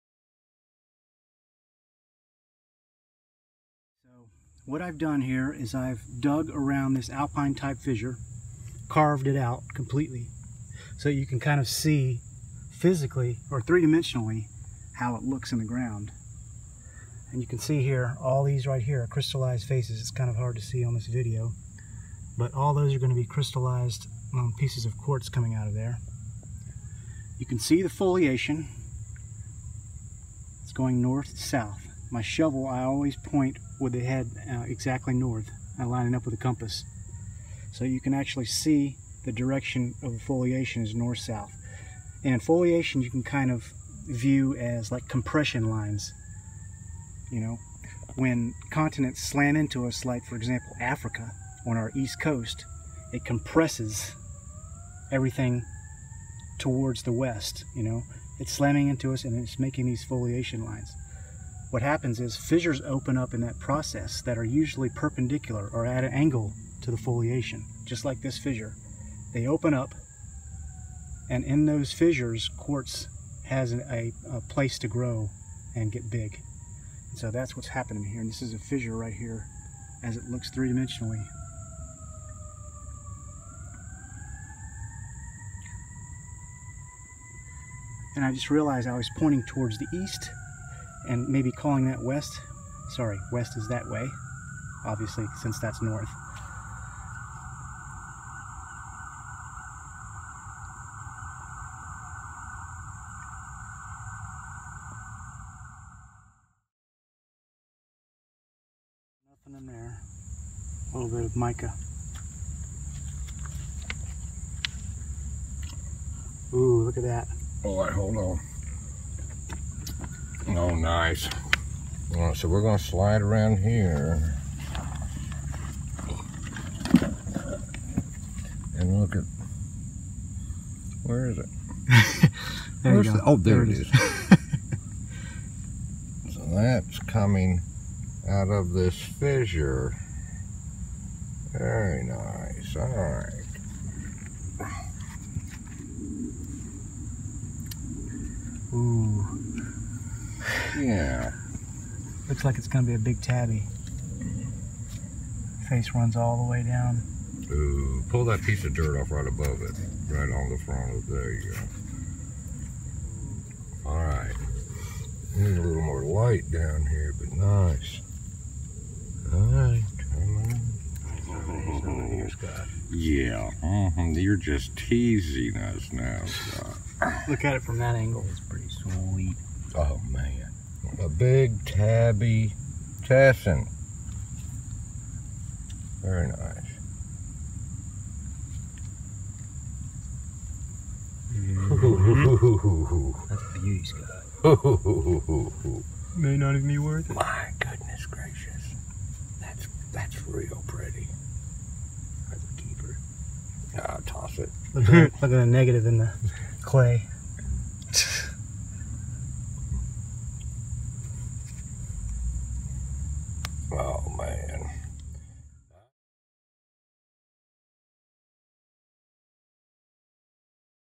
BigTessin
August 9th, 2016. This is an alpine-type fissure I found and excavated at a construction site.  I carved it out so you can see what they look like in 3 dimensions and relative to the foliation.  After a bit of excavating I discover a large beautiful tabular tessin habit quartz crystal.

To support me you can send tips or donations of bitcoin to this address:   1NcYHgkAhpyvWV7s1bsi4twSewtrx4Qq4c